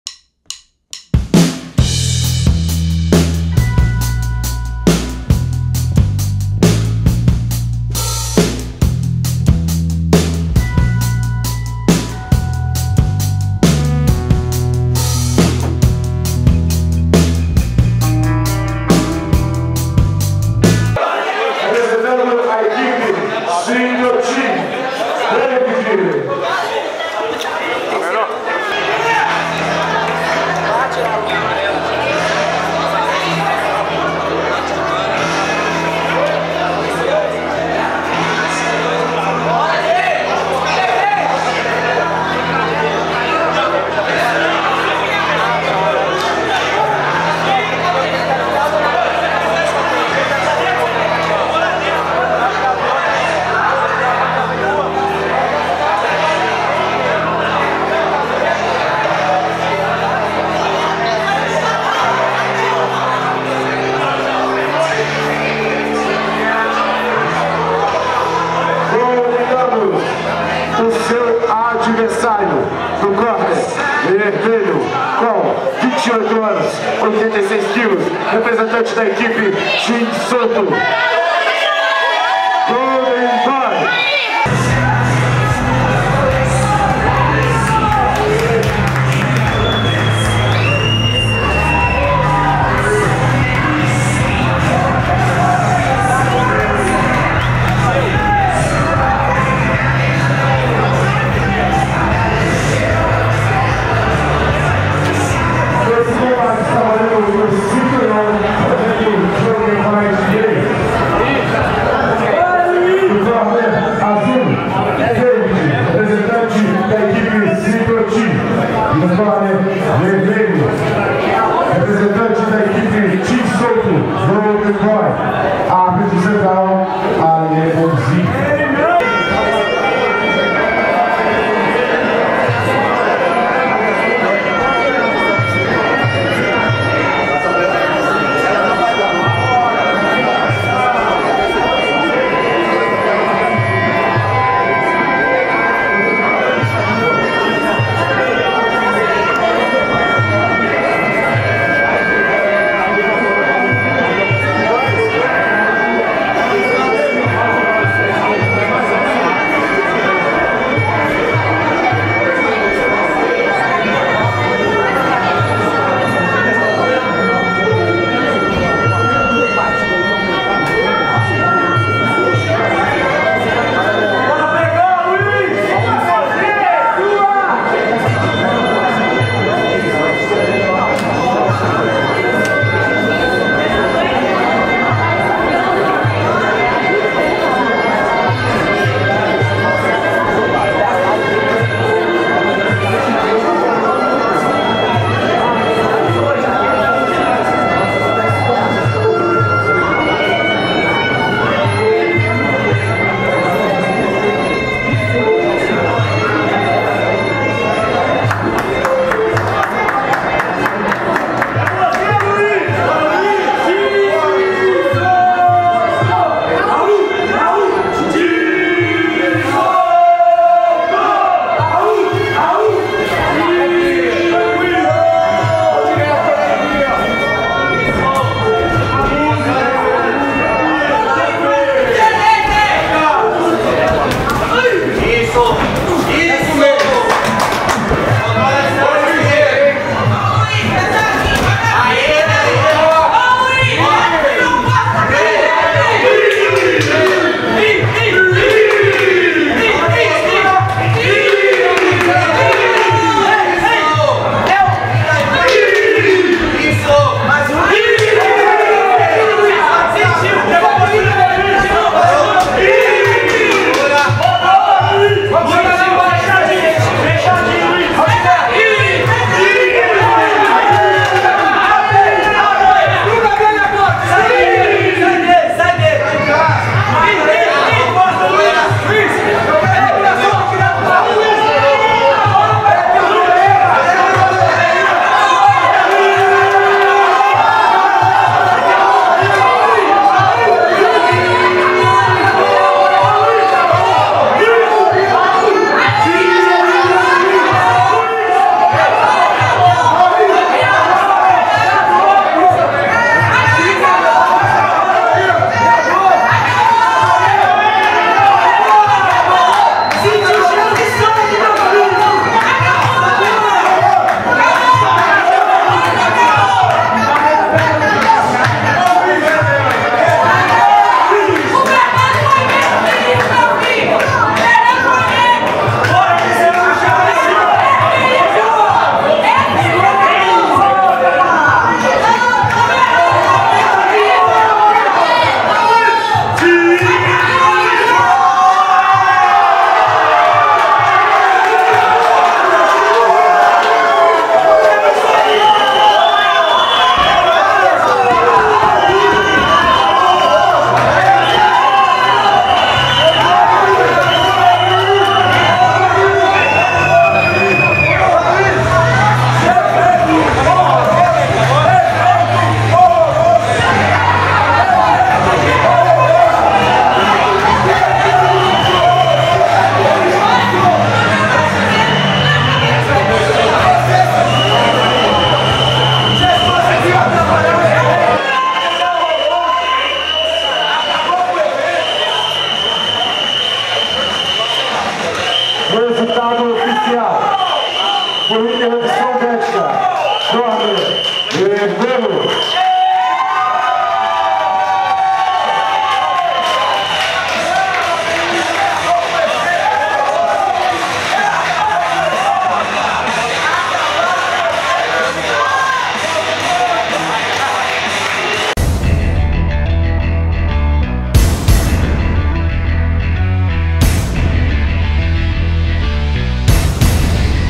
Golden Boy vs David - Joker Fight 3
Na luta principal do Joker Fight 3, Luiz Golden Boy (Team Souto) venceu David (Citrô Team) por interrupção médica no primeiro round.